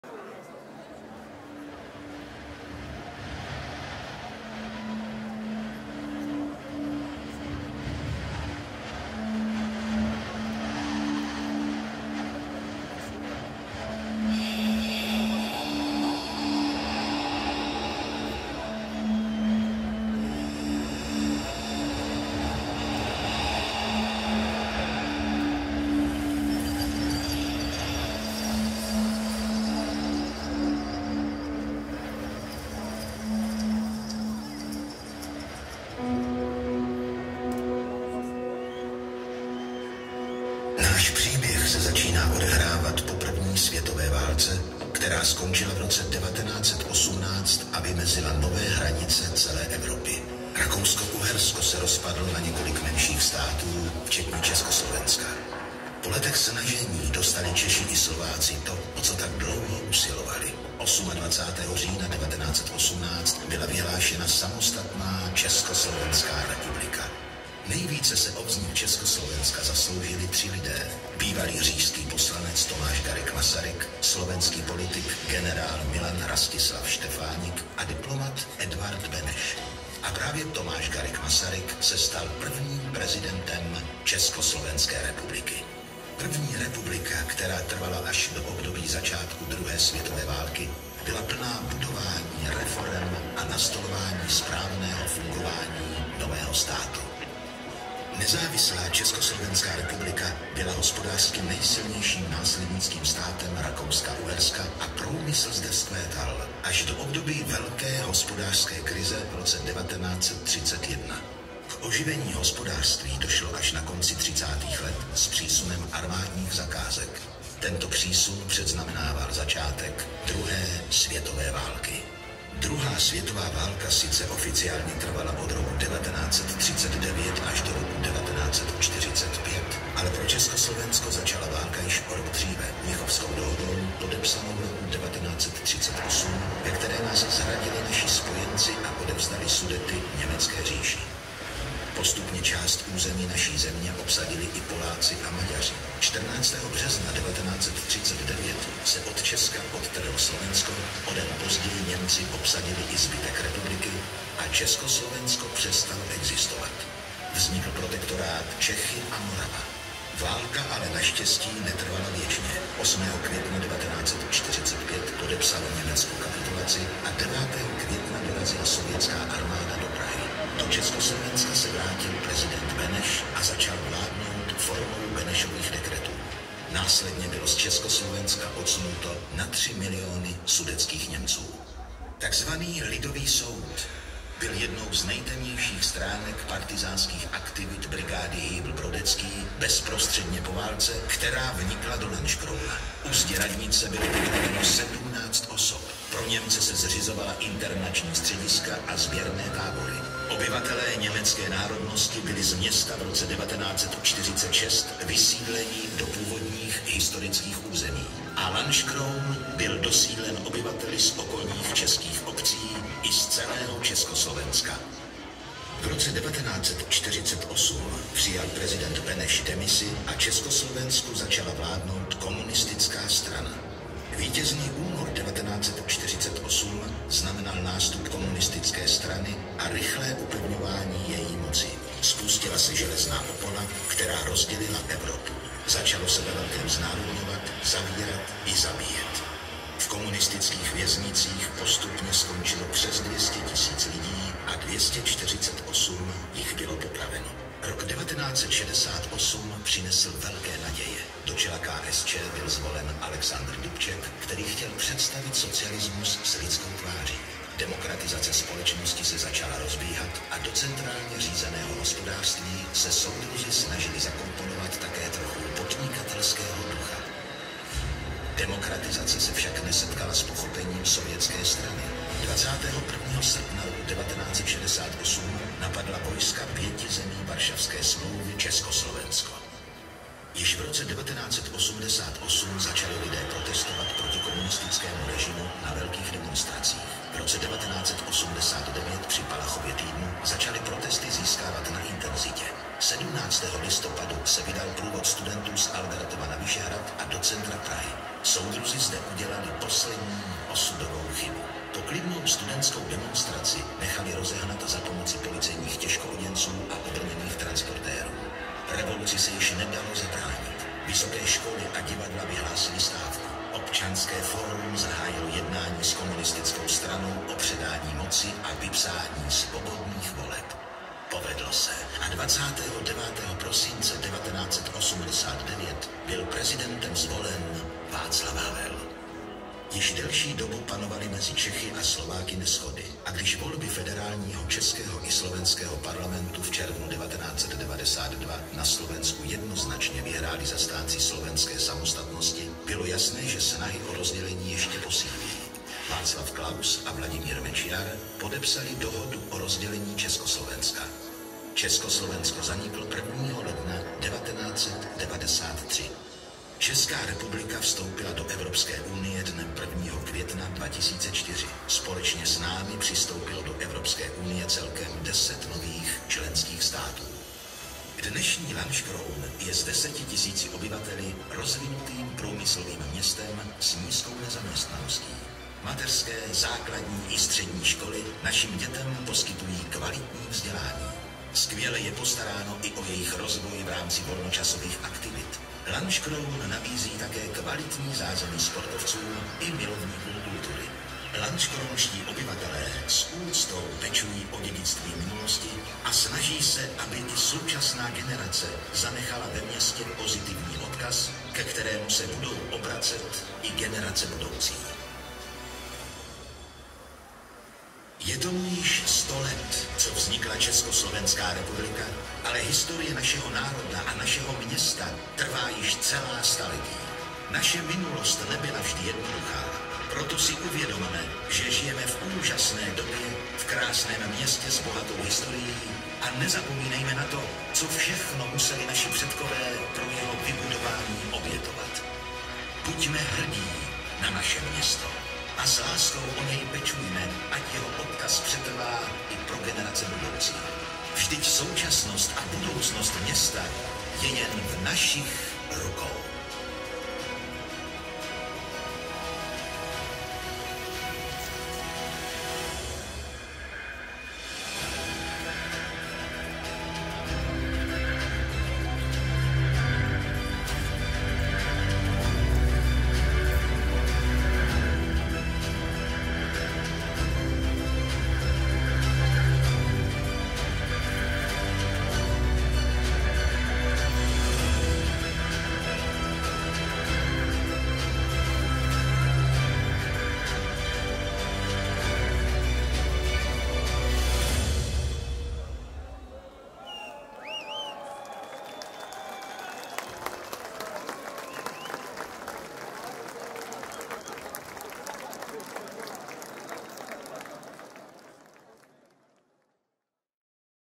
Videomapping Lanškroun 2018
Videomapping ke 100. výročí od vzniku Československa.
videomapping - animace - světelné show - av technika